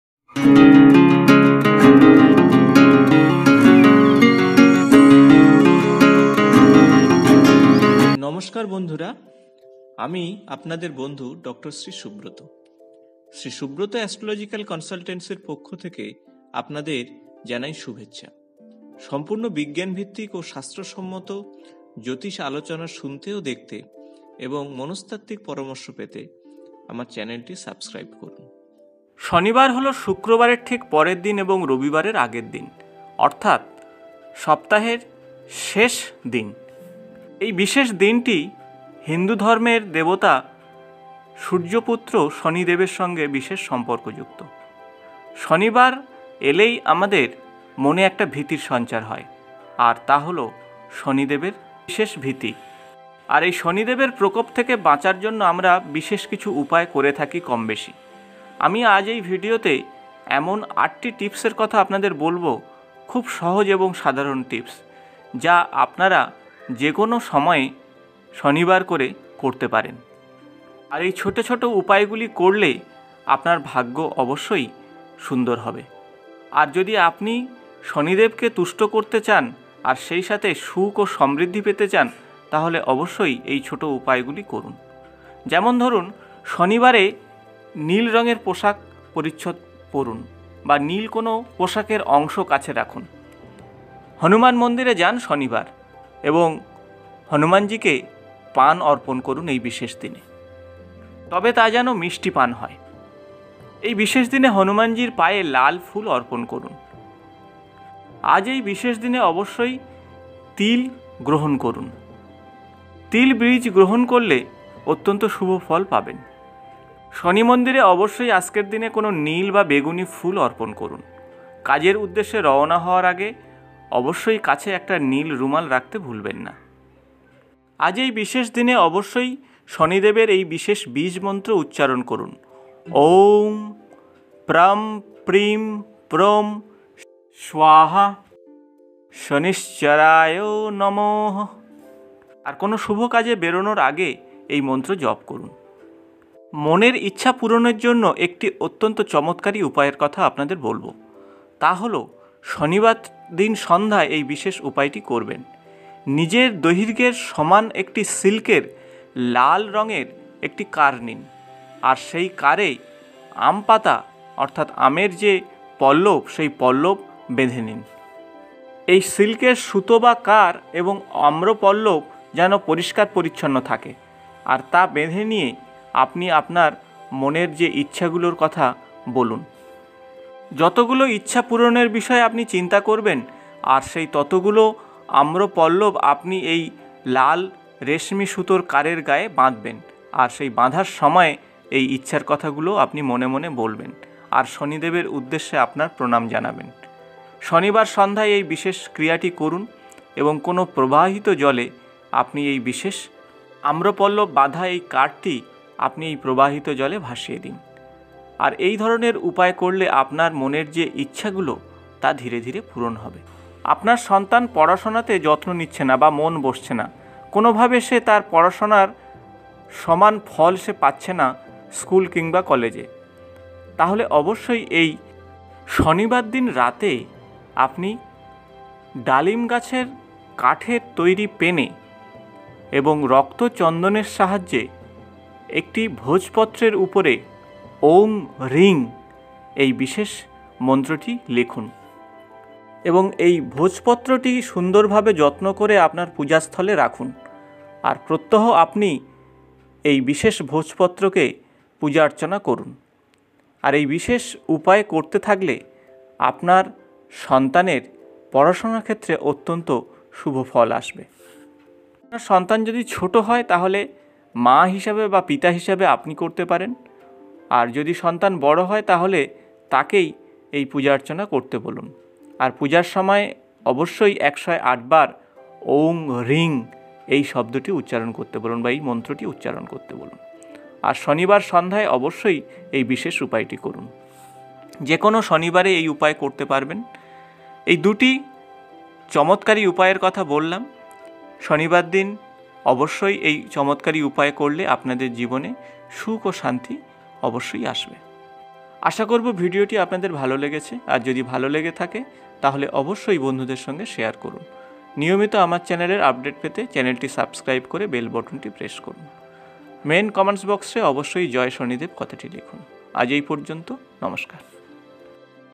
শনিবার সন্ধ্যায় চুপিচুপি লাল রেশমি কারে করুন চমৎকারী উপায়। সকল মনস্কামনা পূর্ণ হবে |DR SHREE SUBRATA
IIHINDU ASTROLOGY & VASTU SHASTRA SECRETS REVEALEDII

ABOUT VIDEO:-

Dr.Shree Subrata, (মনোবিজ্ঞানী,জ্যোতিষ ও আধ্যাত্মিক পরামর্শদাতা)
        JYOTISH SHASTRI,JYOTISH VIDYABIBHUSHAN, VASTU BACHASPATI, Certificate Course on Bhagavad Gita, (ISKCON)

Follow Me,